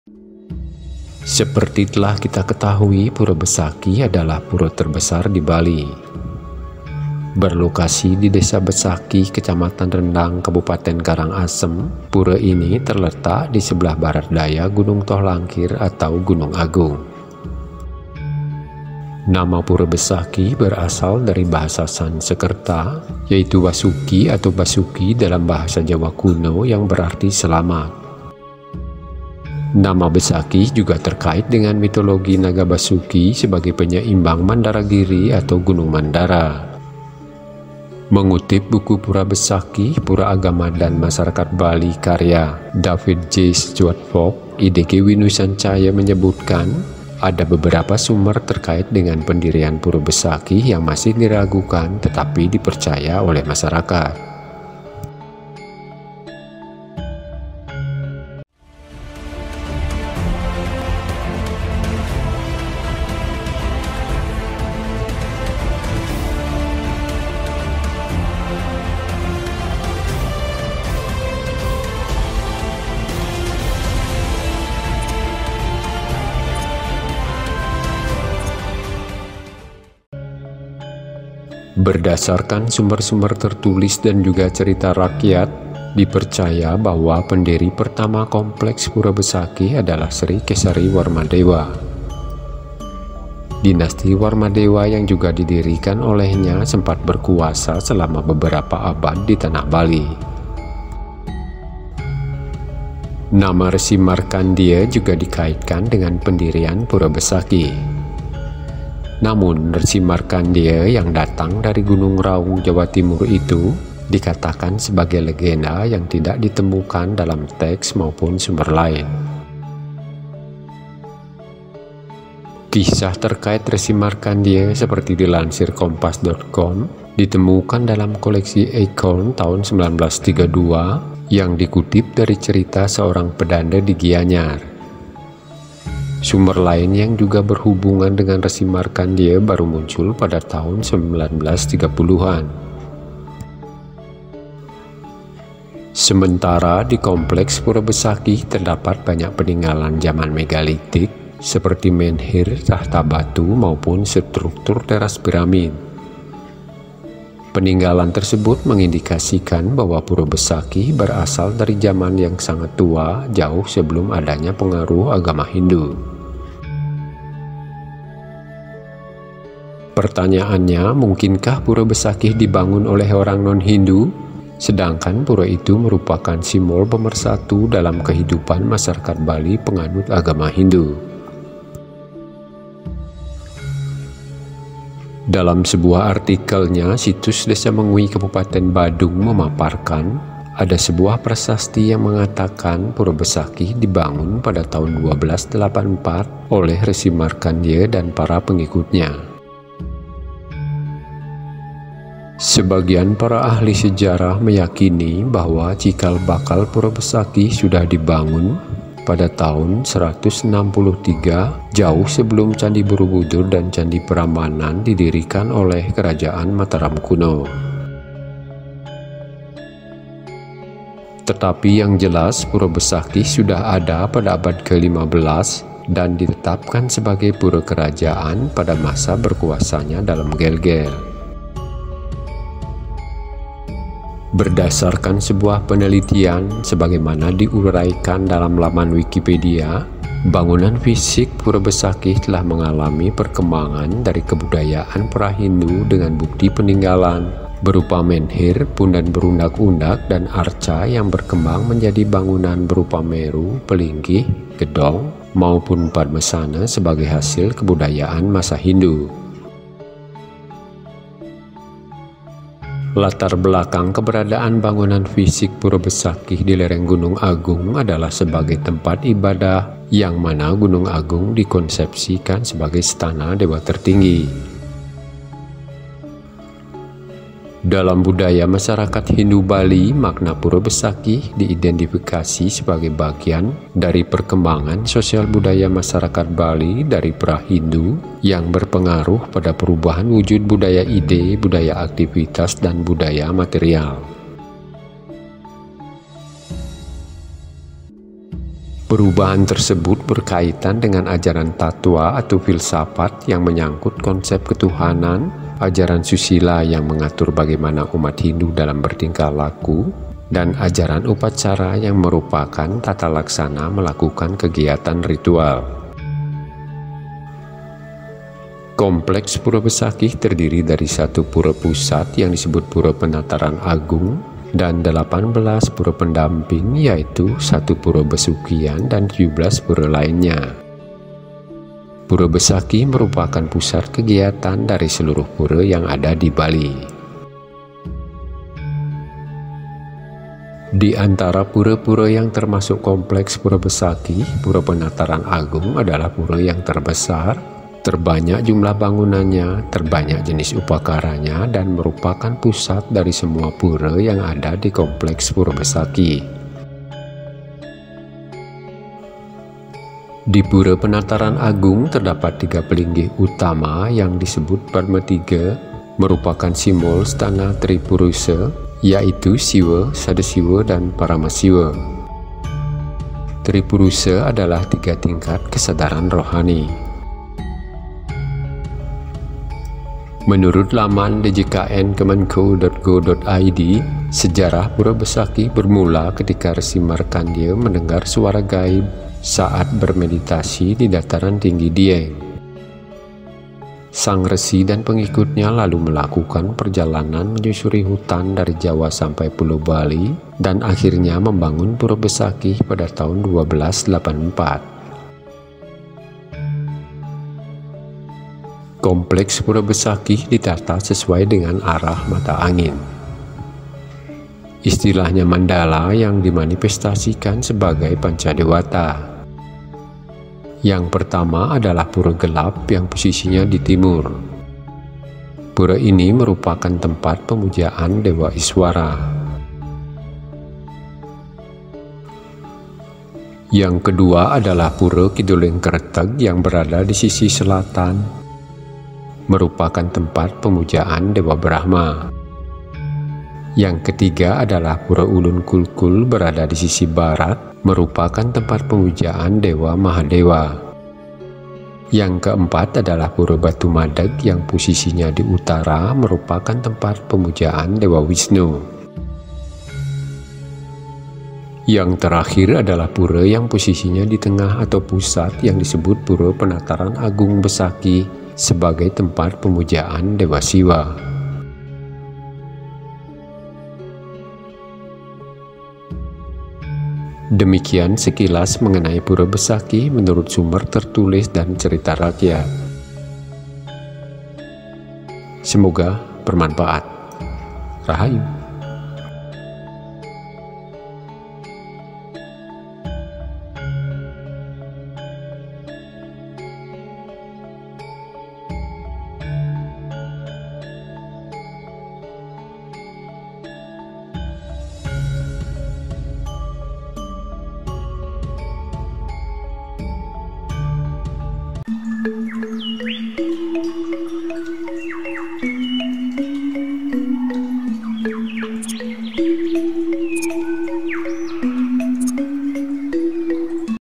Pura Besakih dan Misteri Pendiriannya
Seperti telah kita ketahui, Pura Besakih adalah pura terbesar di Bali.  Berlokasi di Desa Besakih, Kecamatan Rendang, Kabupaten Karangasem, pura ini teretak di sebelah barat daya Gunung Tohlangkir atau Gunung Agung.

Nama Pura Besakih berasal dari bahasa Sansekerta, yaitu wasuki atau basuki dalam bahasa Jawa Kuno yang berarti selamat. Nama Besakih juga terkait dengan mitologi naga Basuki sebagai penyeimbang Mandara Giri atau Gunung Mandara.

Mengutip buku Pura Besakih: Pura Agama dan Masyarakat Bali karya David J. Stuart Fox, IDG Windu Sancaya menyebutkan ada beberapa sumber terkait dengan pendirian Pura Besakih yang masih diragukan tetapi dipercaya oleh masyarakat.

Berdasarkan sumber-sumber tertulis dan juga cerita rakyat, dipercaya bahwa pendiri pertama kompleks Pura Besakih adalah Sri Kesari Warmadewa. Dinasti Warmadewa yang juga didirikan olehnya sempat berkuasa selama beberapa abad di tanah Bali.

Nama Resi Markandya juga dikaitkan dengan pendirian Pura Besakih. Namun, Resi Markandya yang datang dari Gunung Raung, Jawa Timur itu dikatakan sebagai legenda yang tidak ditemukan dalam teks maupun sumber lain.